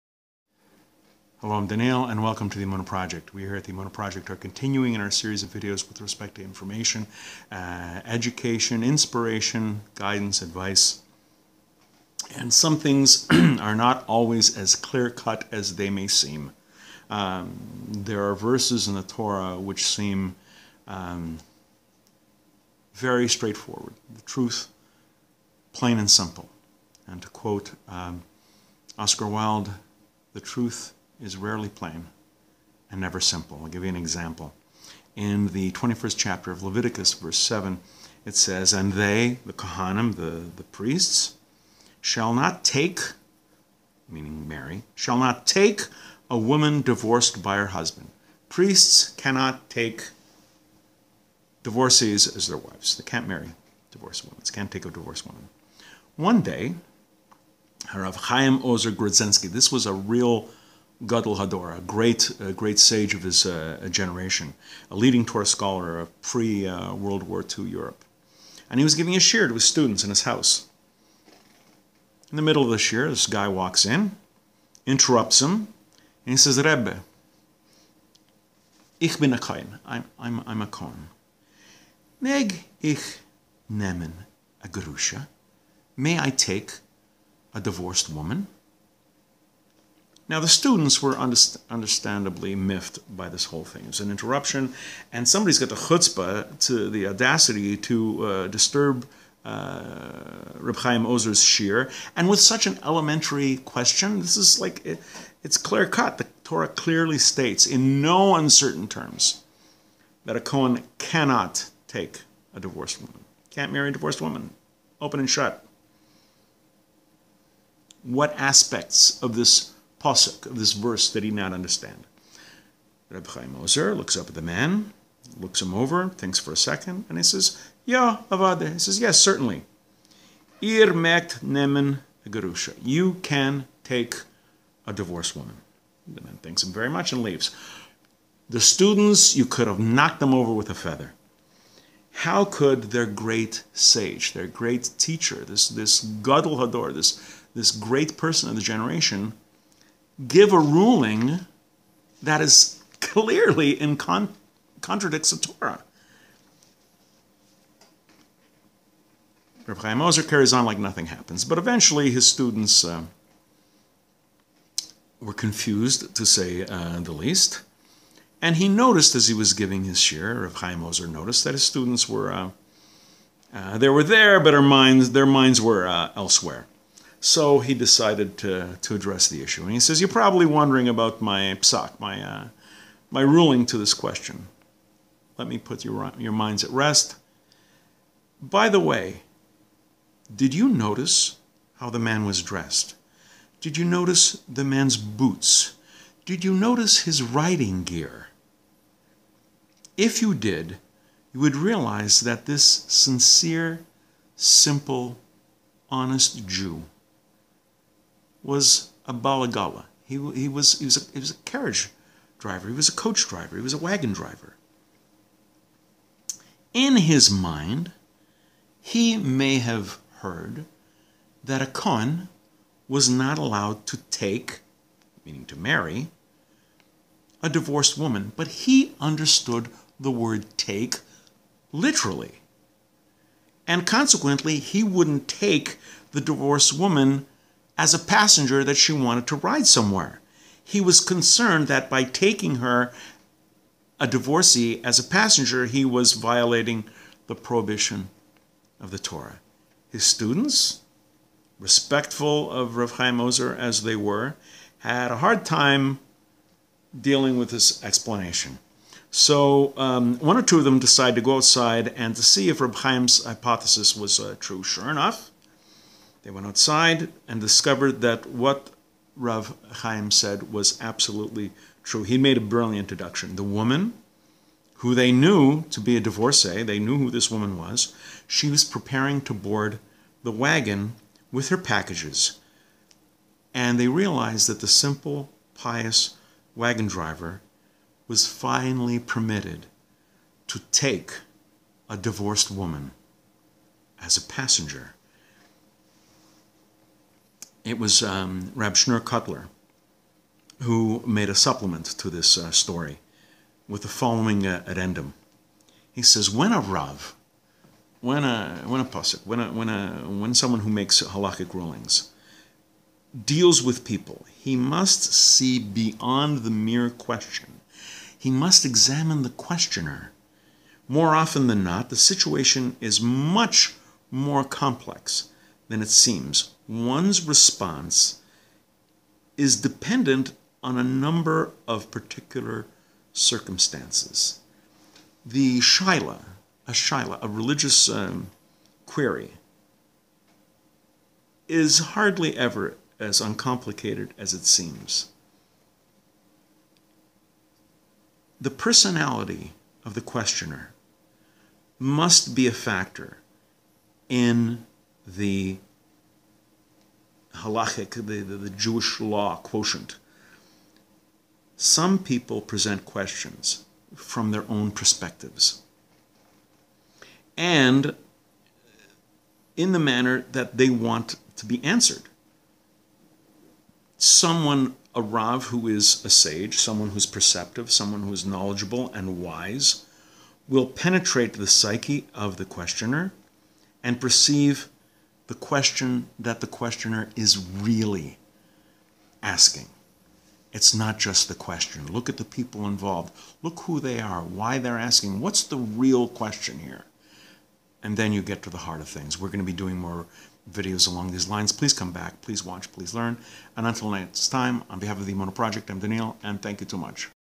The Wagon Driver & the Divorcée
Emunah Project R' Chaim Ozer Grodzinski. Leviticus 21:7. Priests shall not take a divorcee. R' Shneur Kotler.